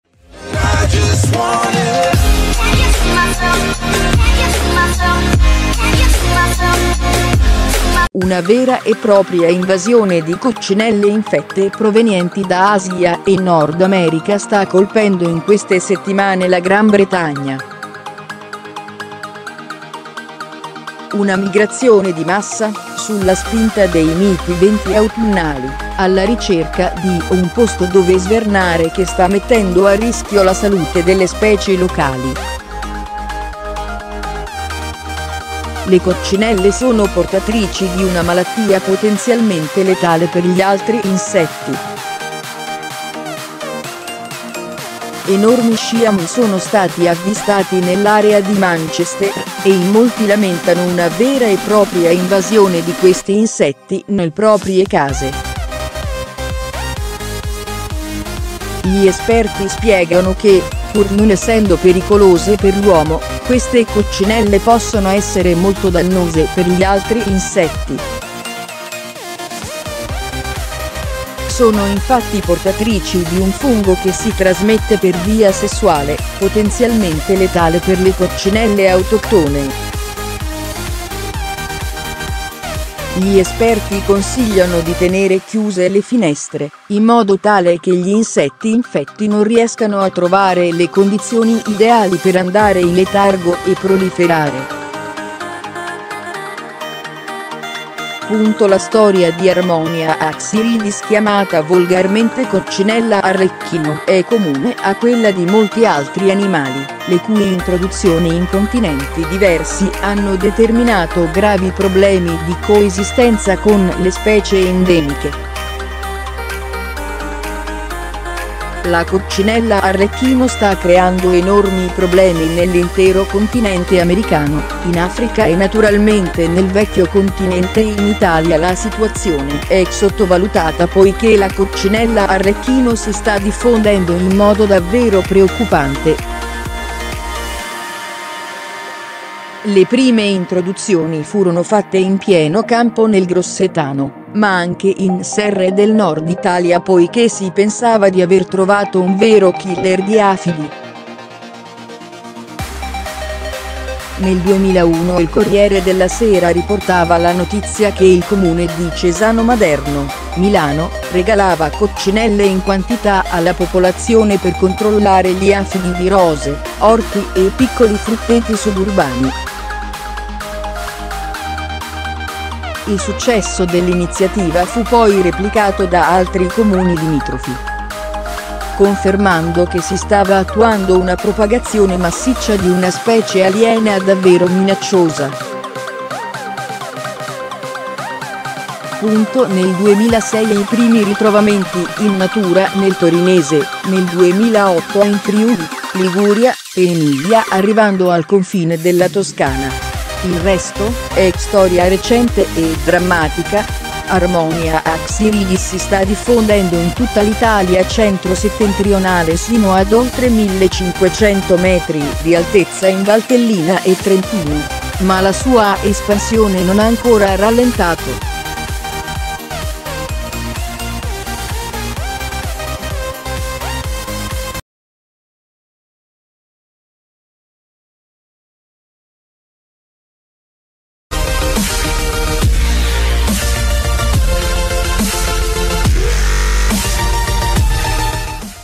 Invasione enorme di coccinelle. L’allarme: “Perché sono pericolose, finestre chiuse” | ULTIMI ARTIC
Una vera e propria invasione di coccinelle infette provenienti da Asia e Nord America sta colpendo in queste settimane la Gran Bretagna. Una migrazione di massa, sulla spinta dei miti venti autunnali, alla ricerca di un posto dove svernare che sta mettendo a rischio la salute delle specie locali. Le coccinelle sono portatrici di una malattia potenzialmente letale per gli altri insetti. Enormi sciami sono stati avvistati nell'area di Manchester e in molti lamentano una vera e propria invasione di questi insetti nel proprie case.Gli esperti spiegano che, pur non essendo pericolose per l'uomo, queste coccinelle possono essere molto dannose per gli altri insetti. Sono infatti portatrici di un fungo che si trasmette per via sessuale, potenzialmente letale per le coccinelle autoctone. Gli esperti consigliano di tenere chiuse le finestre, in modo tale che gli insetti infetti non riescano a trovare le condizioni ideali per andare in letargo e proliferare.
La storia di Harmonia axyridis chiamata volgarmente coccinella arlecchino è comune a quella di molti altri animali, le cui introduzioni in continenti diversi hanno determinato gravi problemi di coesistenza con le specie endemiche. La coccinella arlecchino sta creando enormi problemi nell’intero continente americano, in Africa e naturalmente nel vecchio continenteIn Italia la situazione è sottovalutata poiché la coccinella arlecchino si sta diffondendo in modo davvero preoccupante.
Le prime introduzioni furono fatte in pieno campo nel grossetano ma anche in serre del nord Italia poiché si pensava di aver trovato un vero killer di afidi. nel 2001 il Corriere della sera riportava la Notizia che il Comune di Cesano Maderno (Milano) regalava coccinelle in quantità alla popolazione per controlllare gli afidi di rose, orti e piccoli frutteti suburbani. Il successo dell’iniziativa fu poi replicato da altri comuni limitrofi, confermando che si stava attuando una propagazione massiccia di una specie aliena davvero minacciosa.
Nel 2006 i primi ritrovamenti in natura nel torinese, nel 2008 in Friuli, Liguria, Emilia arrivando al confine della Toscana! Il resto è storia recente e drammatica: Harmonia axyridis si sta diffondendo in tutta l’Italia centro- settentrionale sino ad oltre 1500 m di altezza in Valtellina e Trentino; ma la sua espansione non ha ancora rallentato.